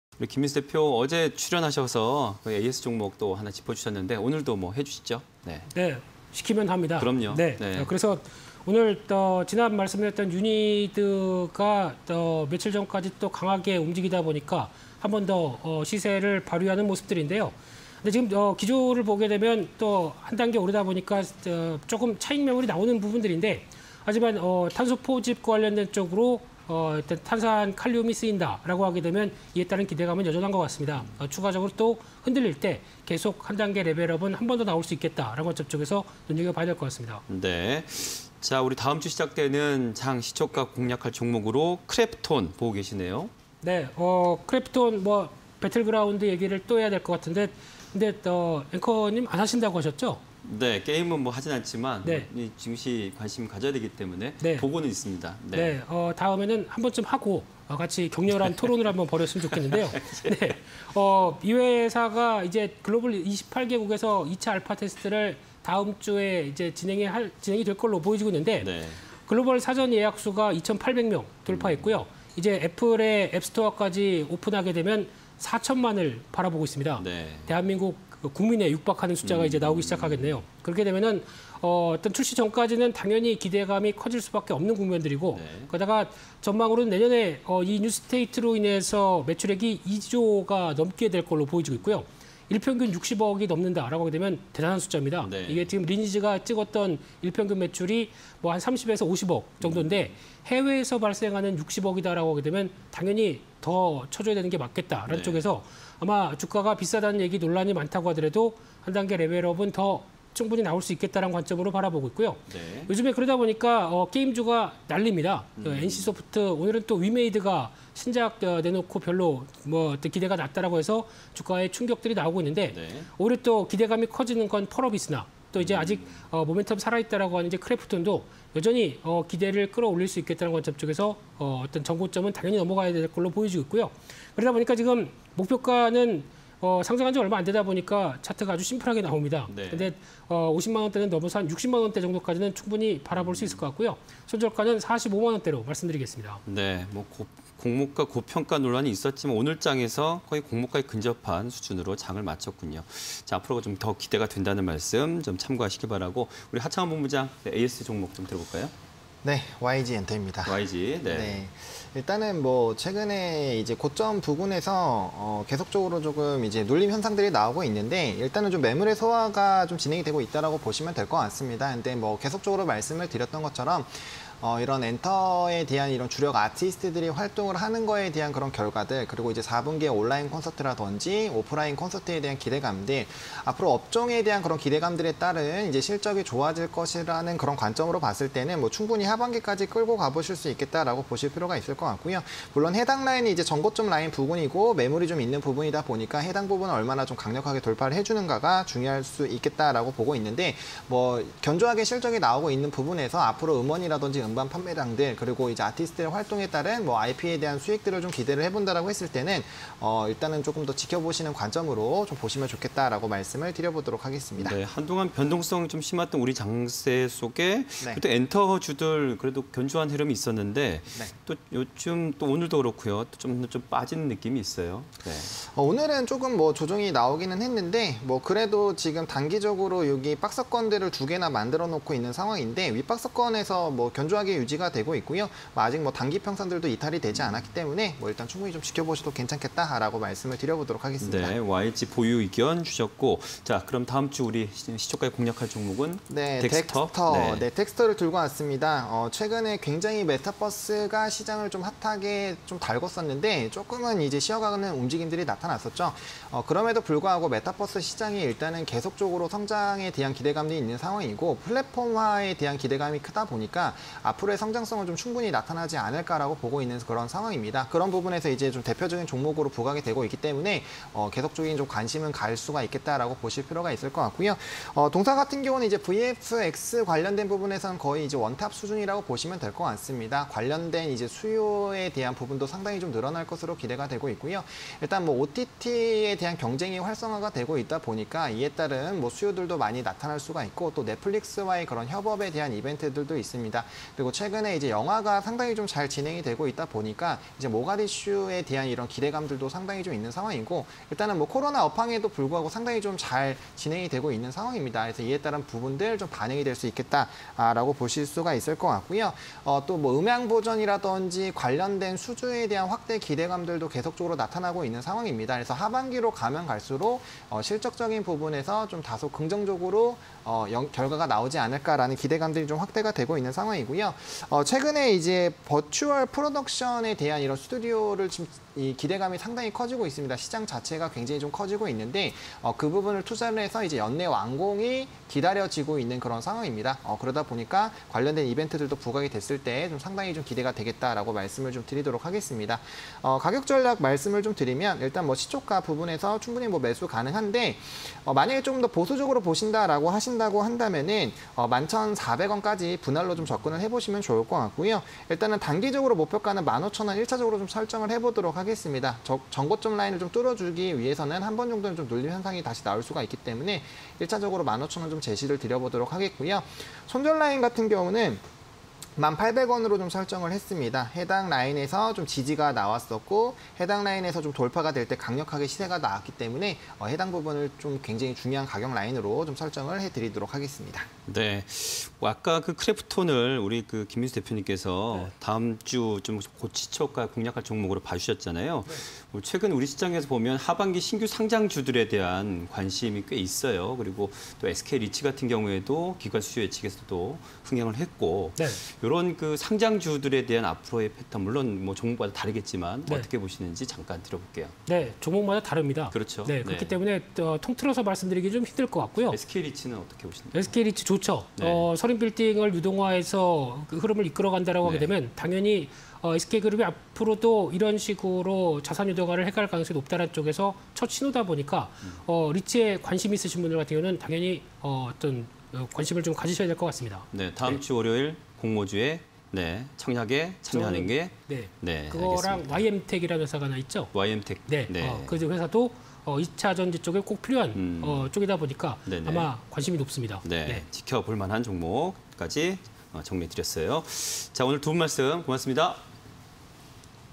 크래프톤... 신작 '배틀그라운드:뉴스테이트' 기대감 / 덱스터... VFX 원스톱 솔루션 보유ㆍOTT 경쟁 확대_내일장 간판주 (20210827)
크래프톤(259960)
- 신작 '배틀그라운드:뉴스테이트' 기대감
- 글로벌 사전예약자 수 2,800만 명 돌파
- '뉴스테이트' 내년 매출액 2.24조 원 예상

덱스터(206560)
- VFX 원스톱 솔루션 보유ㆍOTT 경쟁 확대
- 하반기 영화 시장 회복… 실적 기대감 확대
- '버추얼 프로덕션 스튜디오' 연내 완공 예정